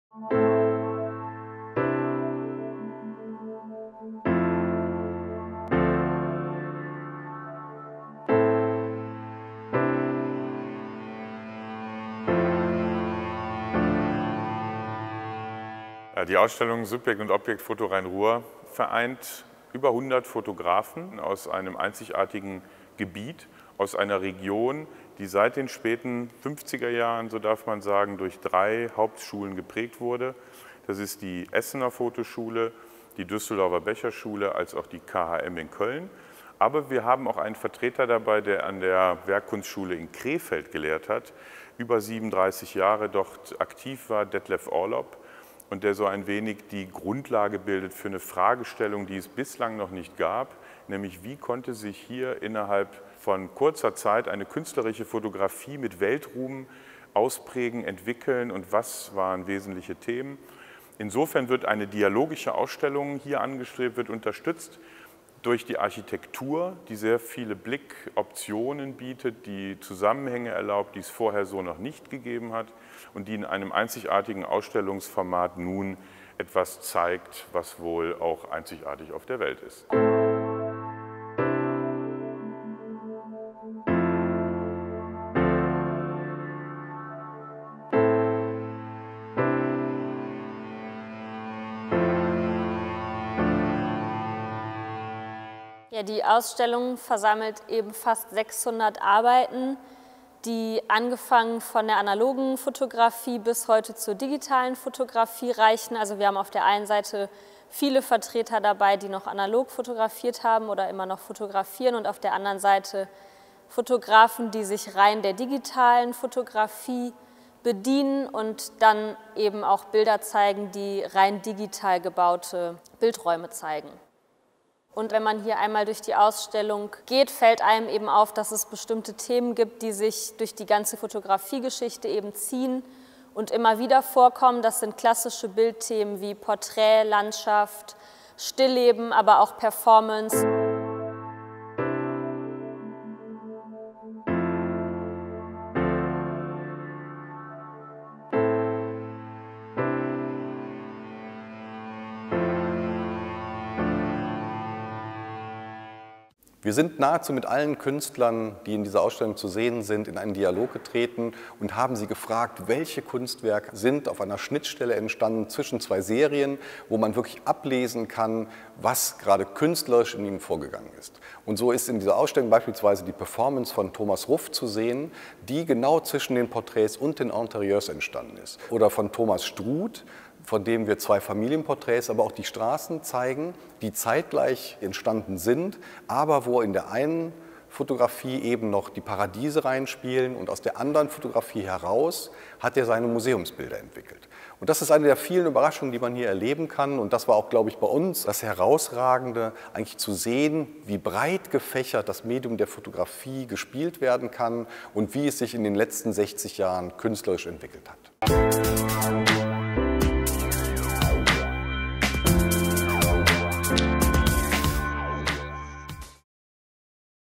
Trailer SUBJEKT und OBJEKT. FOTO RHEIN RUHR
Die Kunsthalle Düsseldorf präsentiert mit „SUBJEKT und OBJEKT. FOTO RHEIN RUHR“ über 100 Positionen und rund 600 Arbeiten der zeitgenössischen Fotografie. Beleuchtet werden die Entwicklung der Fotografie und der wechselseitige Einfluss der Kunsthochschulen in der Metropolregion Rhein Ruhr.

Das Kuratoren-Trio Dana Bergmann, Ralph Goertz und Gregor Jansen stellt  in einem kurzen Einblick das Konzept und einzelne Arbeiten der aktuellen Schau vor.